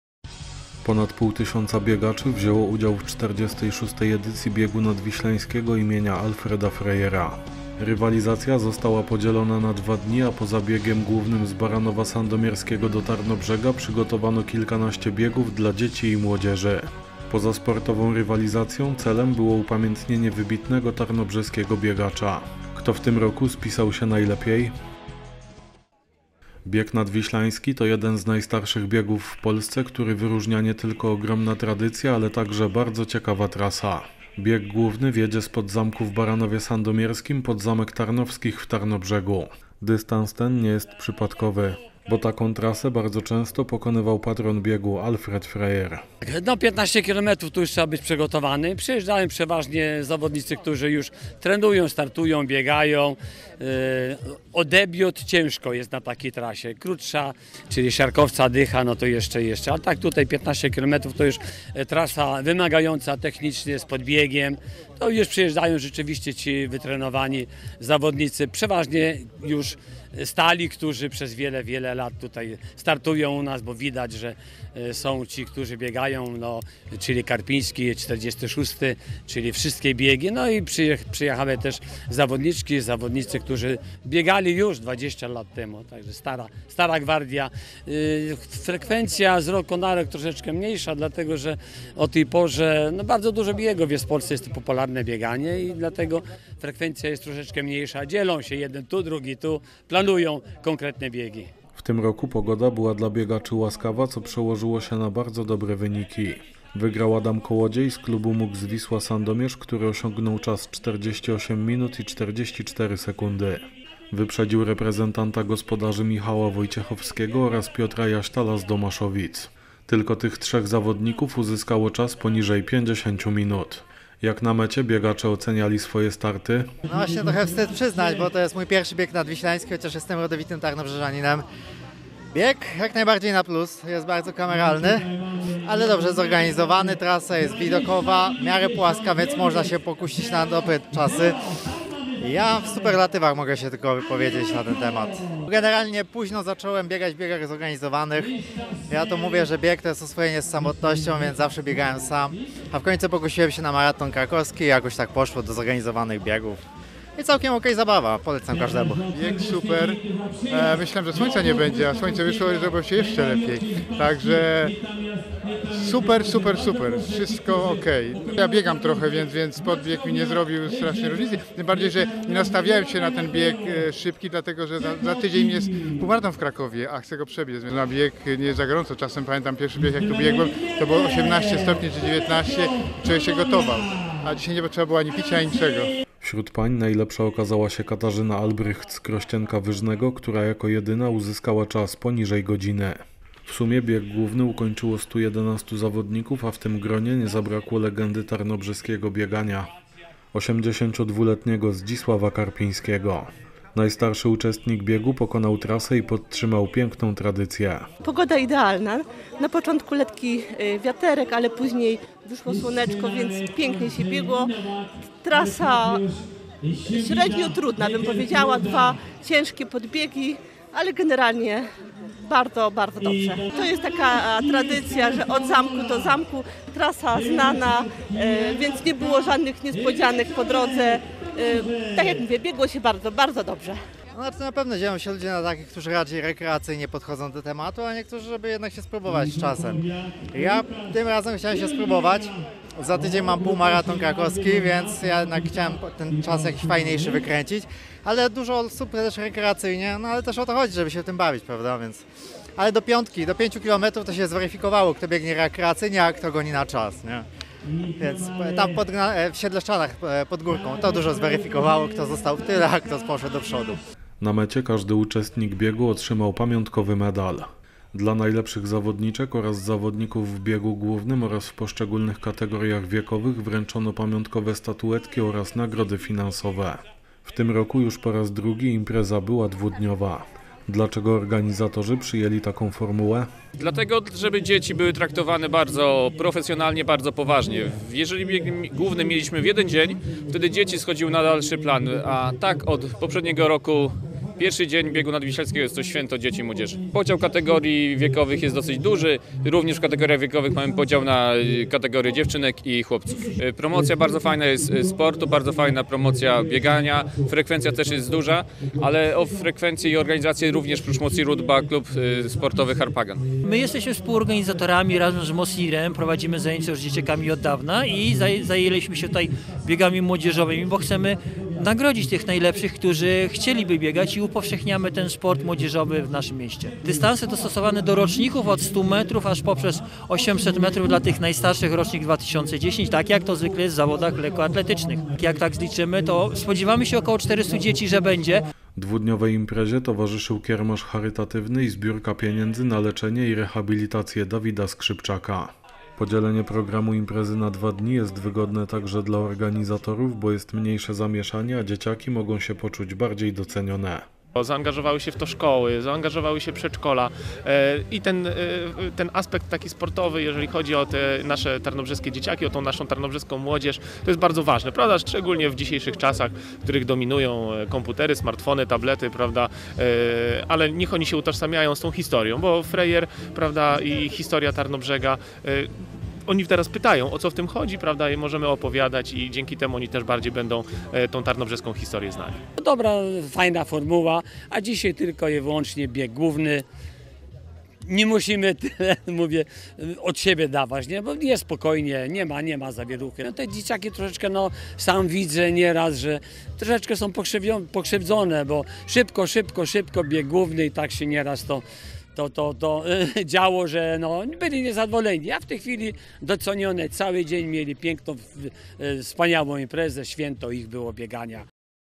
46. BIEG NADWIŚLAŃSKI IMIENIA ALFREDA FREYERA
Ponad pół tysiąca uczestników wzięło udział w 46. edycji Biegu Nadwiślańskiego imienia Alfreda Freyera. Rywalizacja została podzielona na dwa dni a poza biegłem głównym z Baranowa Sandomierskiego do Tarnobrzega przygotowano kilkanaście biegów dla dzieci i młodzieży. Poza sportową rywalizacją celem było upamiętnienie wybitnego tarnobrzeskiego biegacza. Kto w tym roku spisał się najlepiej? Zapraszamy do obejrzenia.